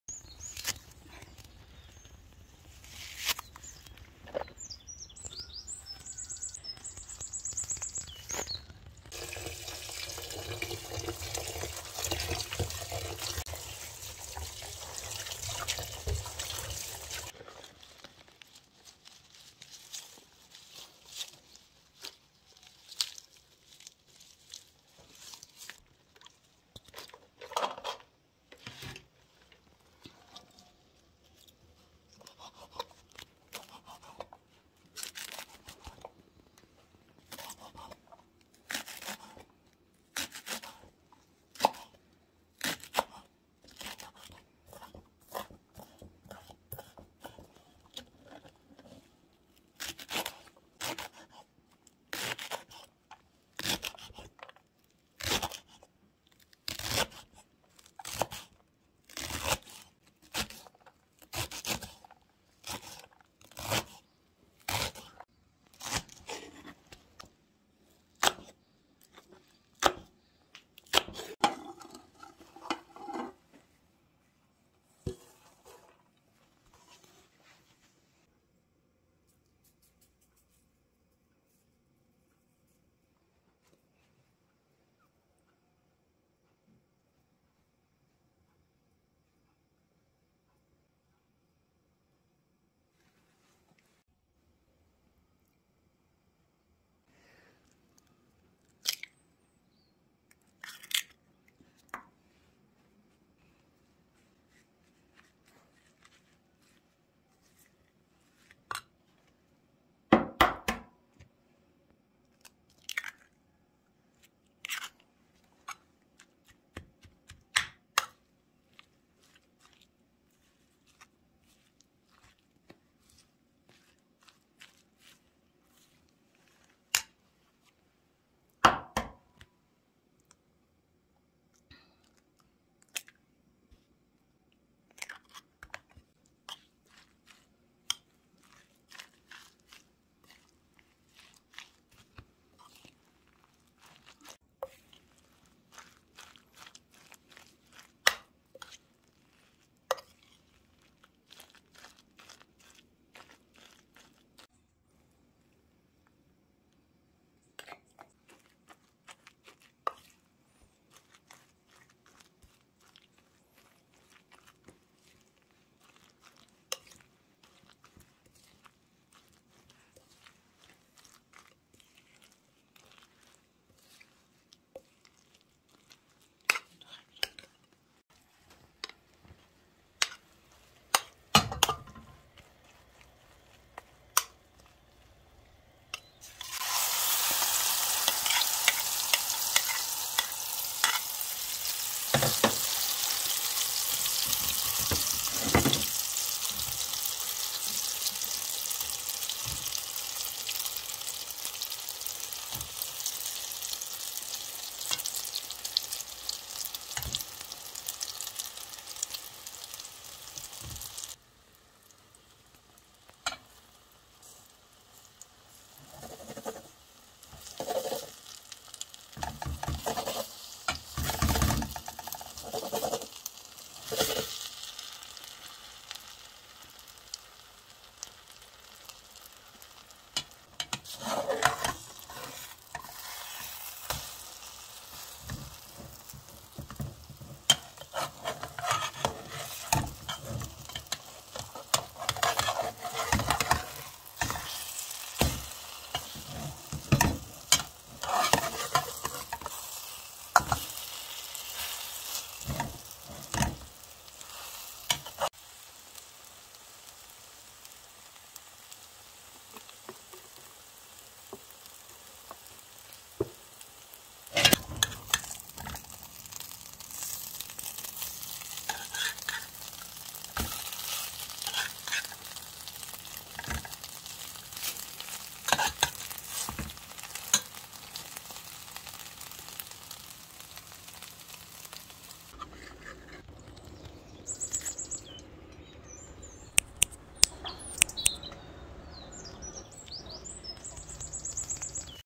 how to make Garlic leaves omelet it's so savoury
Recipes Ingredients :

Garlic leaves
Salt
Pepper
Cinnamon
Turmeric
baking powder
egg
Rice flour